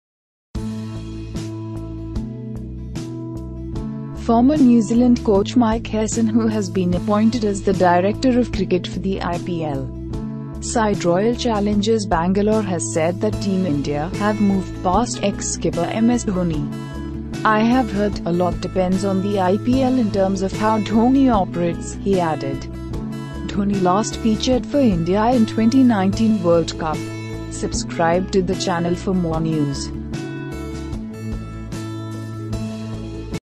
India have moved past MS Dhoni: Ex-NZ coach Mike Hesson
Former New Zealand coach Mike Hesson who has been appointed as the director of cricket for the IPL side Royal Challengers Bangalore has said that Team India "have moved past" ex-skipper MS Dhoni. "I have heard...a lot depends on the IPL in terms of how Dhoni operates," he added. Dhoni last featured for India in 2019 World Cup.